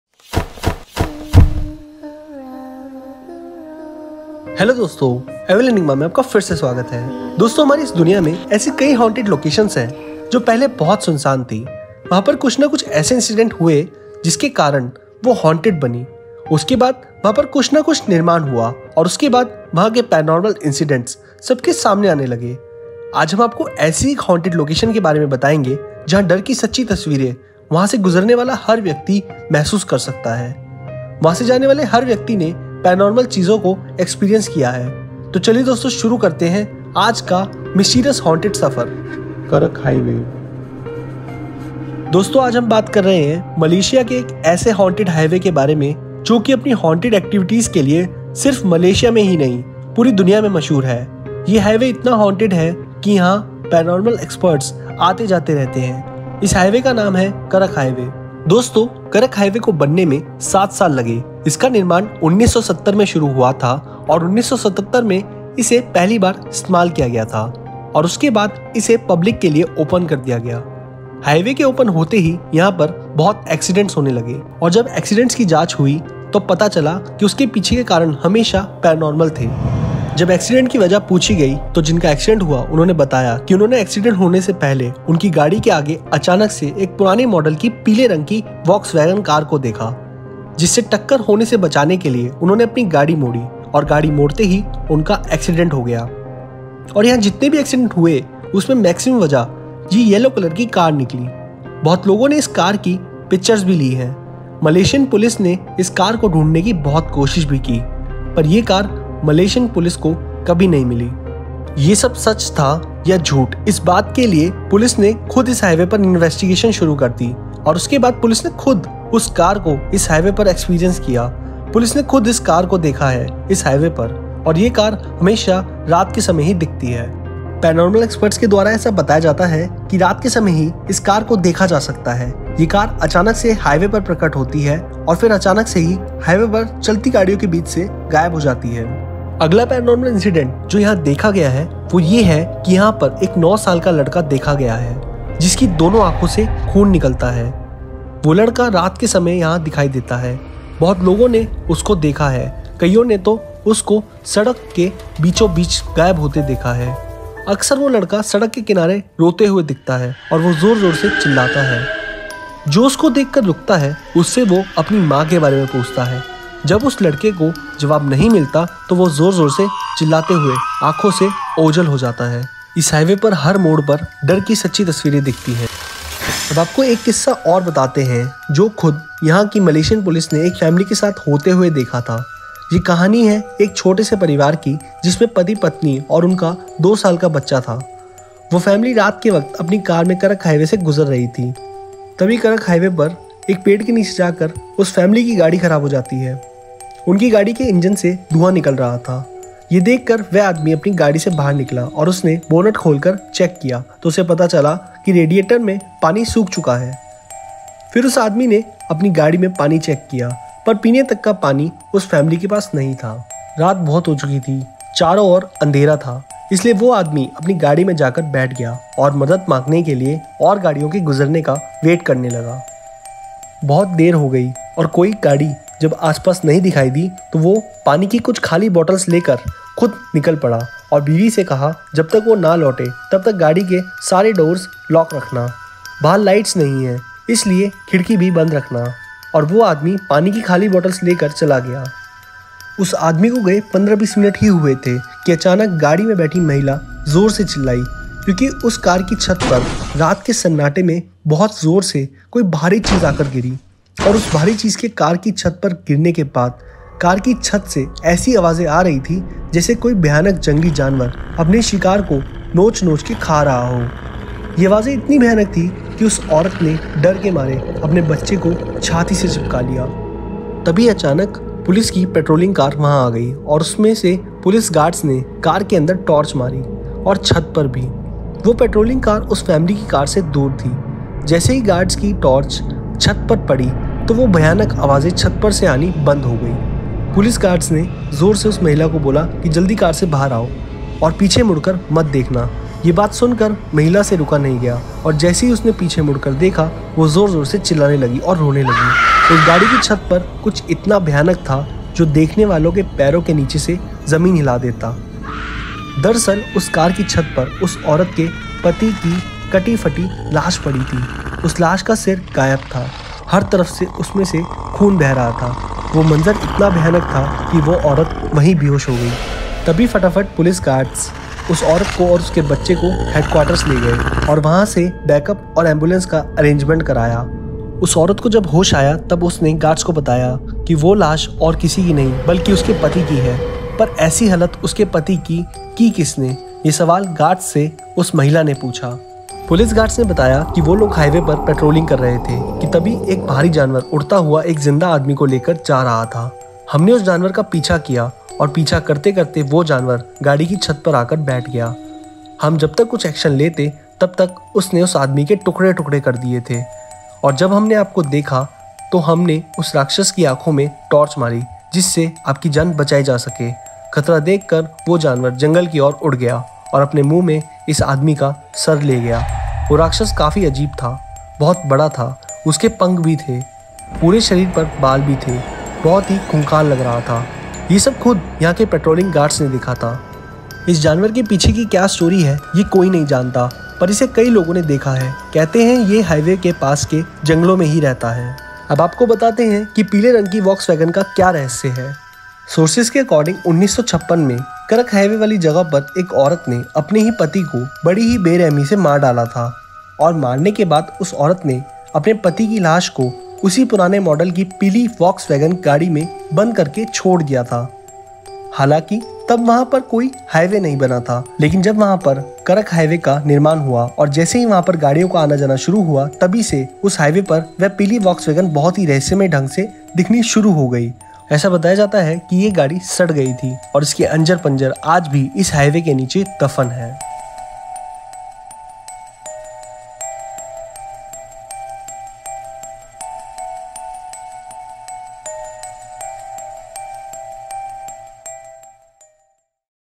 दुनिया का सबसे भूतिया Highway | KARAK HIGHWAY Story | Horror Hindi Story - Haunted Yellow Car
Iss video me jaane duniya ke sabse haunted highway ke baare me.
Yeh highway hai KARAK Highway, Lebuhraya Karak, Malaysia.

Sightings seen on करक हाईवे:
Volkswagen Yellow Beatle
A crying child
Mysterious Behemoth (Unknown Creature)